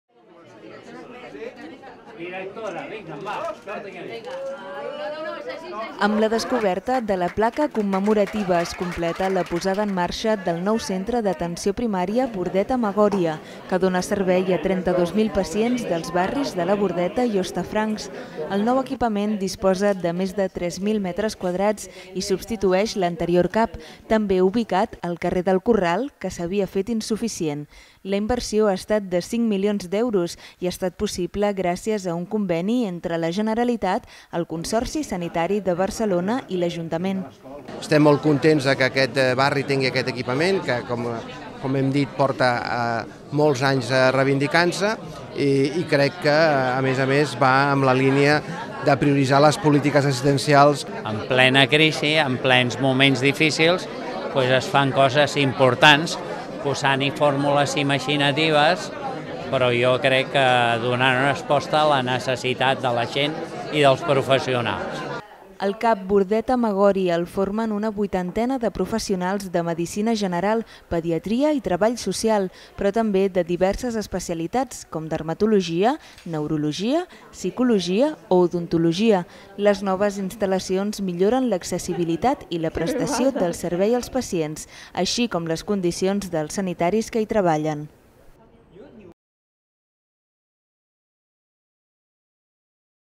Els barris de la Bordeta i Hostafrancs estrenen nou CAP
Situat al carrer del Corral, disposa de 3.000 metres quadrats i dóna servei a més de 30.000 pacients. Més informació